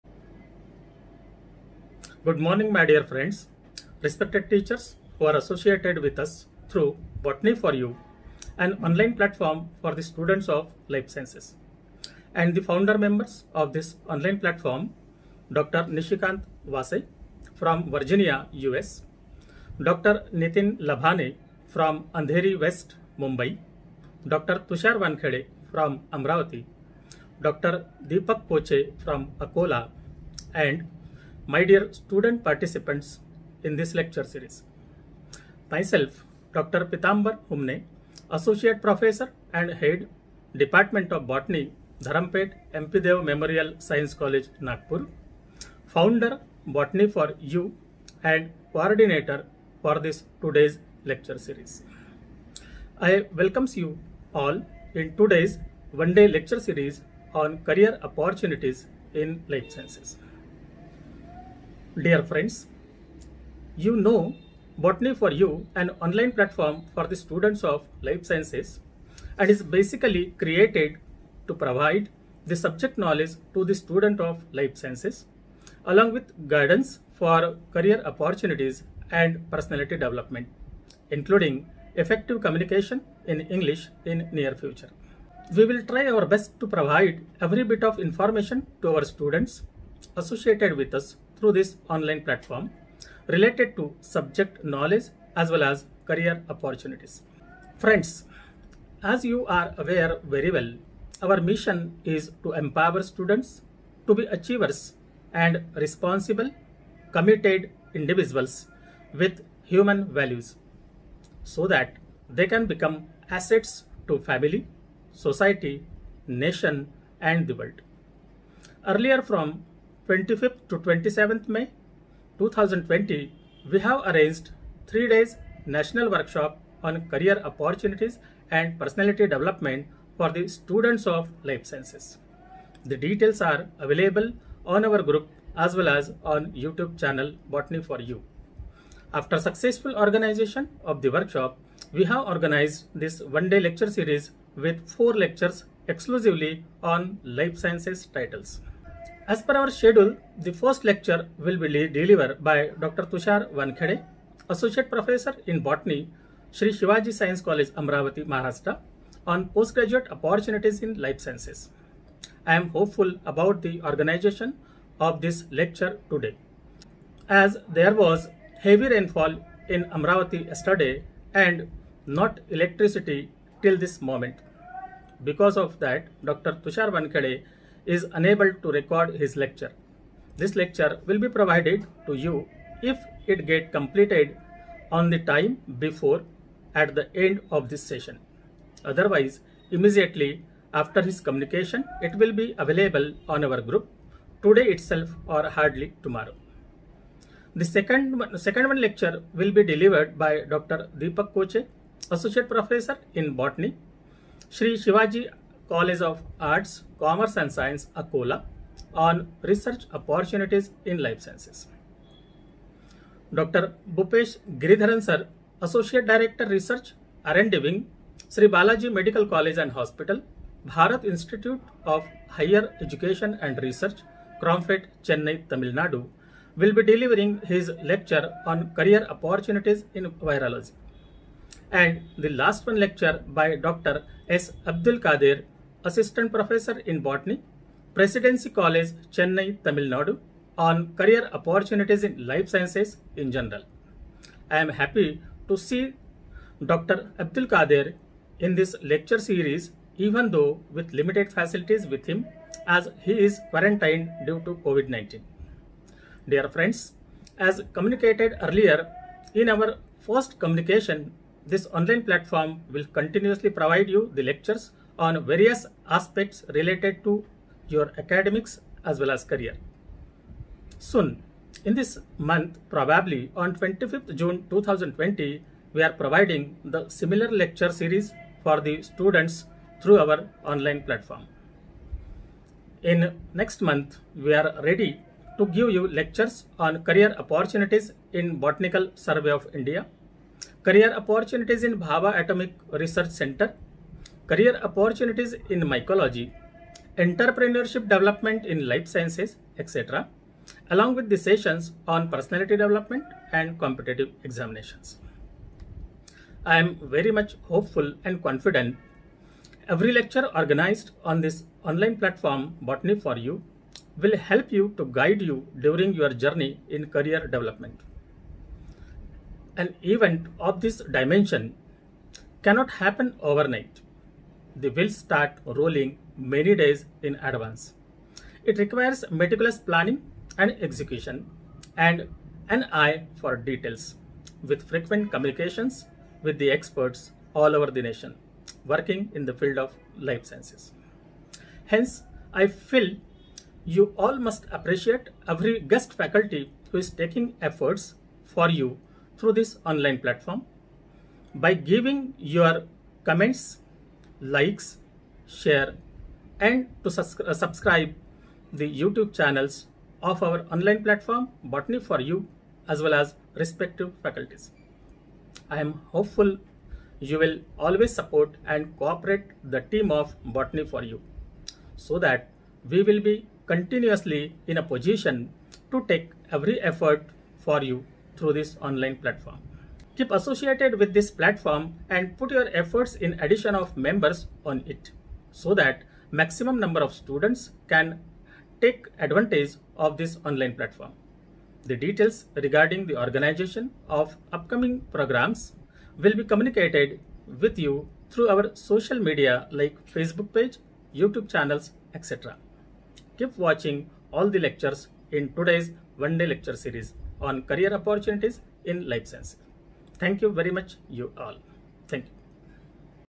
BOTANY 4U | PITAMBAR HUMANE | INTRODUCTORY | 11th JUNE 2020
Introductory speech by DR PITAMBAR HUMANE, Founder, BOTANY 4U & Associate Professor and Head, Department of Botany, Dharampeth M. P. Deo Memorial Science College, Nagpur-33 (MS) on Occasion of ONE DAY LECTURE SERIES ON 'CAREER OPPORTUNITIES IN LIFE SCIENCES' held on 11th JUNE, 2020